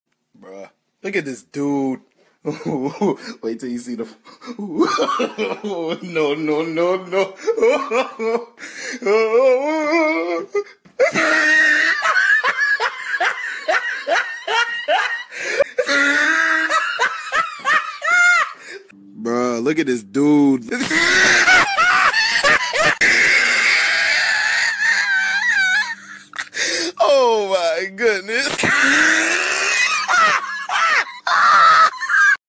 IDW Shadow the Hedgehog in a Nutshell
They made Shadow so dumb in IDW bro, they massacred my boy 😭🤡💀